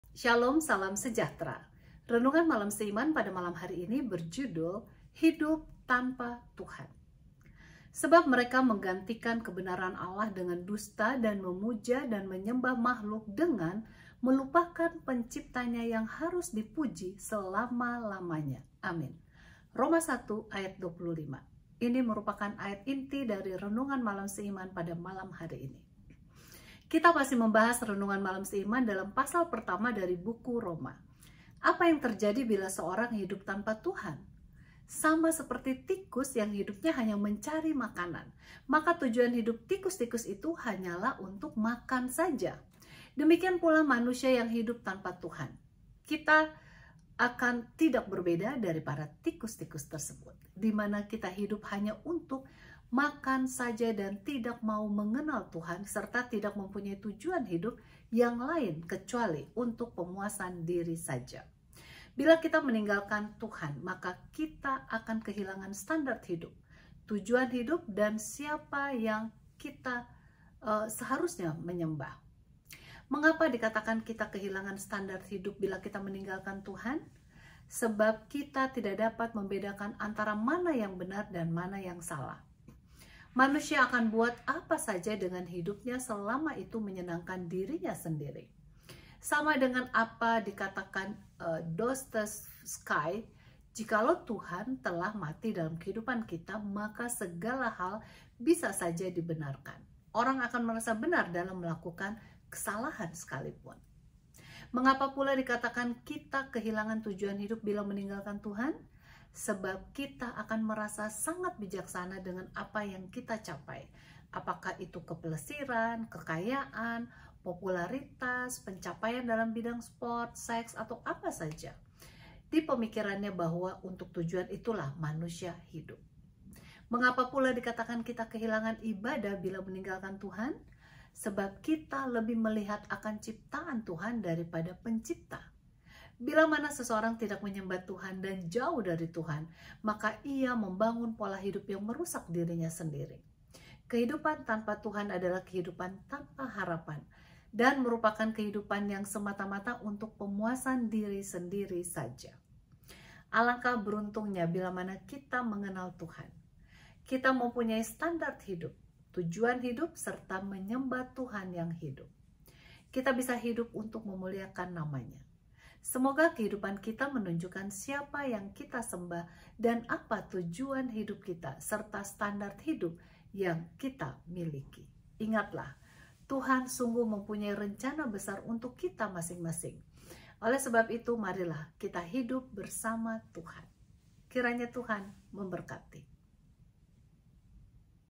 1914 - HIDUP TANPA TUHAN | Renungan Malam Seiman
RMS @peggyiskandarwowor
Penulis: dr. Kathleen Liwidjaja Kuntaraf & Pdt. DR. Jonathan Kuntaraf

HIDUP TANPA TUHAN

“Sebab mereka menggantikan kebenaran Allah dengan dusta dan memuja dan menyembah makhluk dengan melupakan Penciptanya yang harus dipuji selama-lamanya. Amin. ”Roma 1:25

Kita masih membahas renungan malam SEIMAN dalam pasal pertama dari buku Roma. Apa yang terjadi bila seorang hidup tanpa Tuhan? Sama seperti tikus, yang hidupnya hanya mencari makanan, maka tujuan hidup tikus- tikus itu hanyalah untuk makan saja. Demikian pula manusia yang hidup tanpa Tuhan. Kita akan tidak berbeda dari para  tikus tikus tersebut, dimana kita hidup hanya untuk makan saja dan tidak mau mengenal Tuhan, serta tidak mempunyai tujuan hidup yang lain kecuali untuk pemuasan diri saja. Bila kita meninggalkan Tuhan, maka kita akan kehilangan standard hidup, tujuan hidup dan siapa yang kita menyembah seharusnya.

Mengapa dikatakan kita kehilangan standard hidup bila kita meninggalkan Tuhan? Sebab kita tidak dapat membedakan antara mana yang benar dan mana yang salah. Manusia akan buat apa saja dengan hidupnya selama itu menyenangkan dirinya sendiri. Sama dengan apa dikatakan Dostoevsky: “Jikalau Tuhan telah mati dalam kehidupan kita, maka segala hal bisa saja dibenarkan.” Orang akan merasa benar dalam melakukan kesalahan apapun.

Mengapa pula dikatakan kita kehilangan tujuan hidup bila meninggalkan Tuhan? Sebab kita akan merasa sangat bijaksana dengan apa yang kita capai, apakah itu kepelesiran, kekayaan, populeritas, pencapaian dalam bidang sport, seks atau apa saja. Dipemikirannya bahwa untuk tujuan itulah manusia hidup.

Mengapa pula dikatakan kita kehilangan Ibadah bila meninggalkan Tuhan? Sebab kita lebih melihat akan ciptaan Tuhan daripada Pencipta. Bilamana seorang tidak menyembah Tuhan, dan jauh dari Tuhan, maka ia membangun pola hidup yang merusak dirinya sendiri. Kehidupan tanpa Tuhan adalah kehidupan tanpa harapan; dan merupakan kehidupan yang semata-mata untuk pemuasan diri sendiri saja.

Alangkah beruntungnya bilamana kita mengenal Tuhan. Kita mempunyai standard hidup, tujuan hidup serta menyembah Tuhan yang hidup. Kita bisa hidup untuk memuliakan namaNya. Semoga kehidupan kita menunjukkan siapa yang kita sembah, dan apa tujuan hidup kita, serta standard hidup yang kita miliki. Ingatlah, Tuhan sungguh mempunyai rencana besar untuk kita masing-masing oleh sebab itu marilah kita hidup bersama Tuhan.